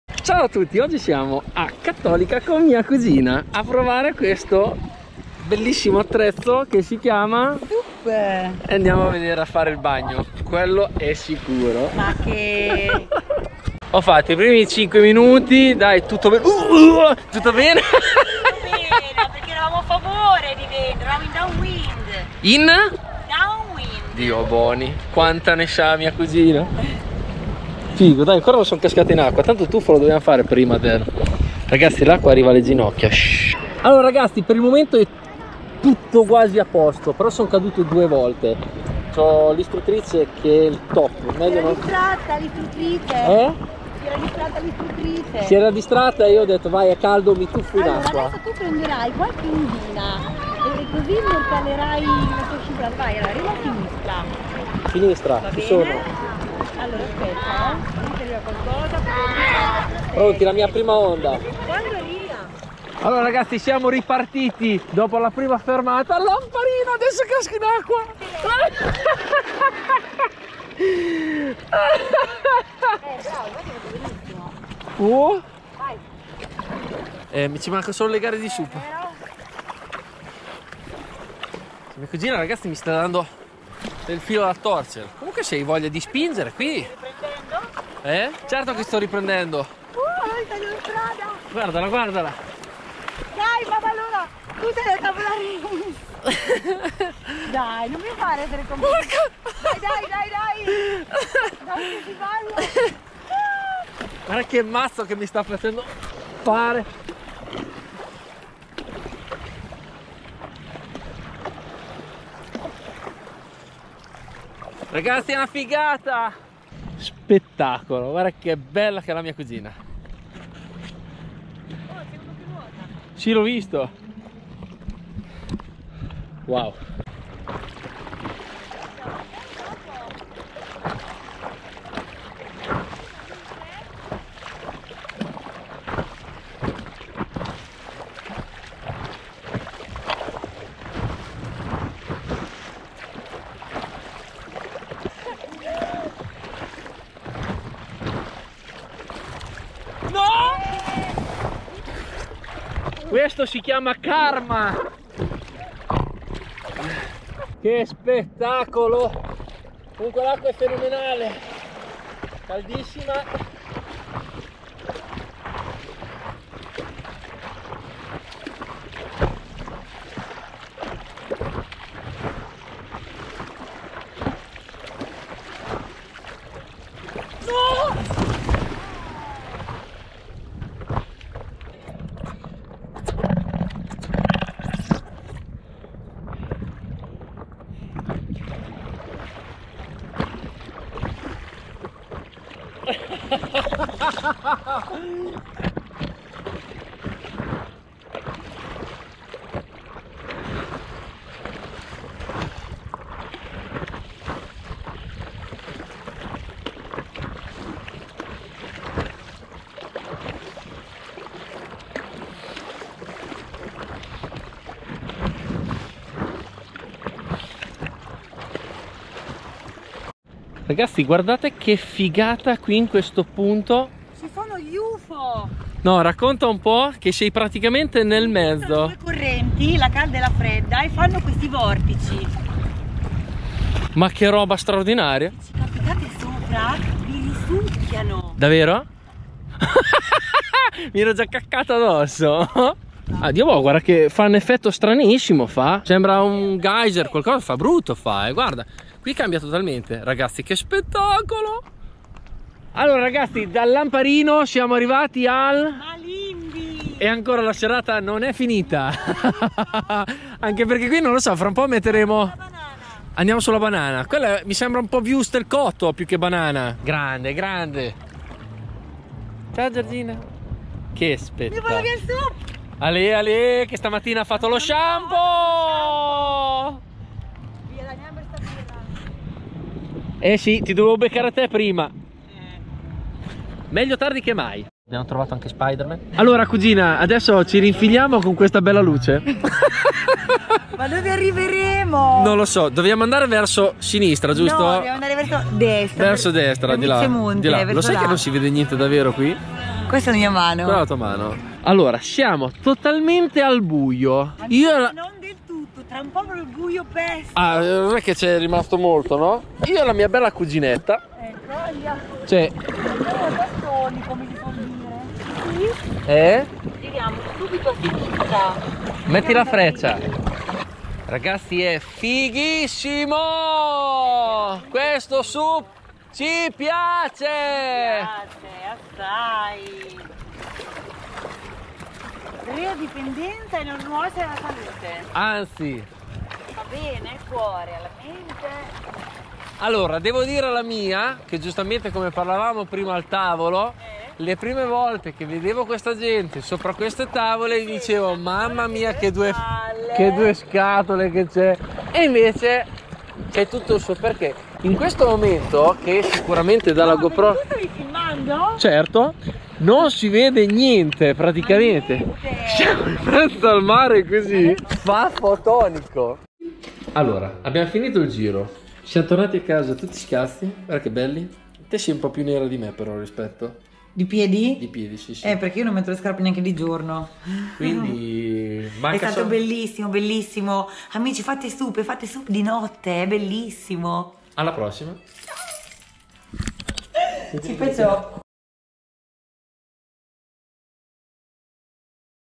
La mia prima volta !!! Sup!!!
La mia prima volta!! SUP!!! la mia prima volta sul sup!! con una coach d'eccezione!!
divertentissimo e faticoso quanto basta!!
bellissimo andare alla scoperta del mio mare con delle super meduse dai colori fantastici!!!